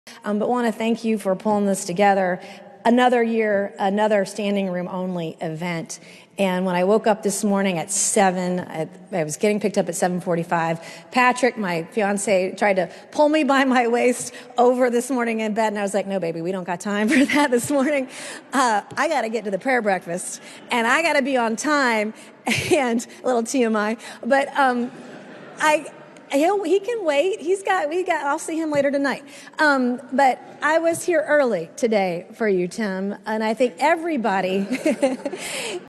Republicans are bashing Nancy Mace for her "TMI" comments...
At Sen. Tim Scott's (R-SC) prayer breakfast, Rep. Nancy Mace (R-SC) told a suggestive "joke" that did not go over well with conservatives who are now bashing the Republican for her "raunchy" remarks.

“When I woke up this morning at 7 — I was getting picked up at 7:45 — Patrick, my fiancé, tried to pull me by my waist over this morning in bed,” Mace said at the prayer breakfast.

While many people laughed at the light-hearted joke many on the right took it to the extreme.